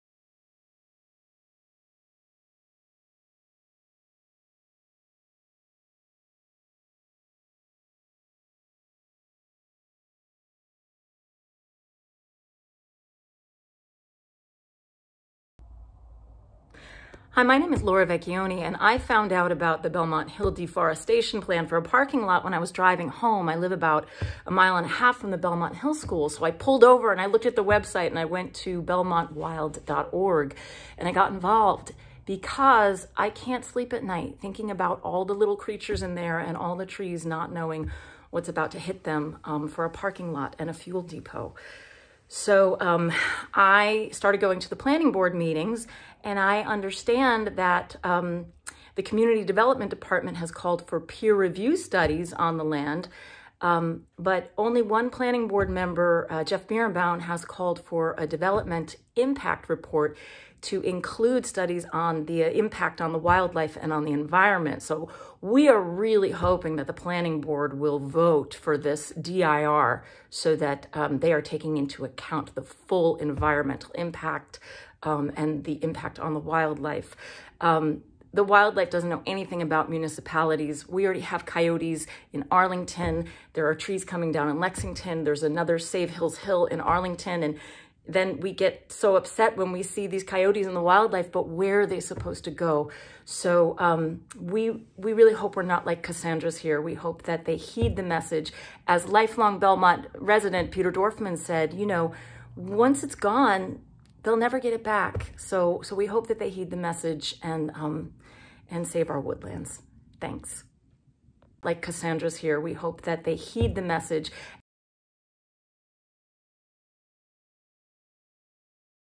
Speak Out Belmont: Laura Vecchione on the Belmont Hill project
Laura Vecchione of BelmontWild.org speaks out about the proposed project by Belmont Hill School to build a parking lot and fuel station.